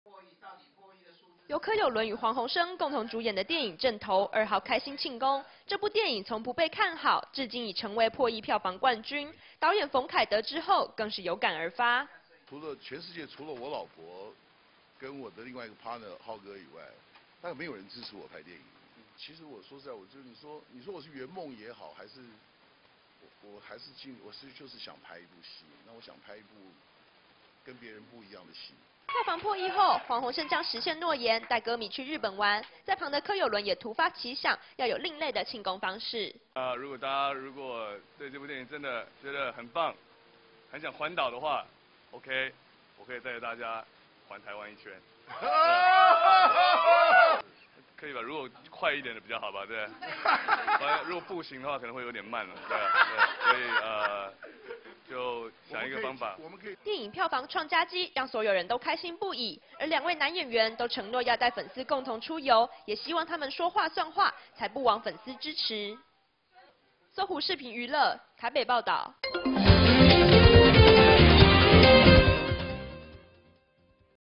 20120203［搜狐］《阵头》票房成冠军 柯有伦庆功比创意（小鬼黃鴻升）
由柯有伦与黄鸿升共同主演的电影「阵头」2号开心庆功，这部电影从不被看好至今已成为破亿票房冠军，导演冯凯得知后更是有感而发。票房破亿后黄鸿升将实现诺言带歌迷去日本玩，在旁的柯有伦也突发奇想，要有另类的庆功方式。电影票房创佳绩让所有人都开心不已，而两位男演员都承诺要带粉丝共同出游，也希望他们说话算话，才不枉粉丝支持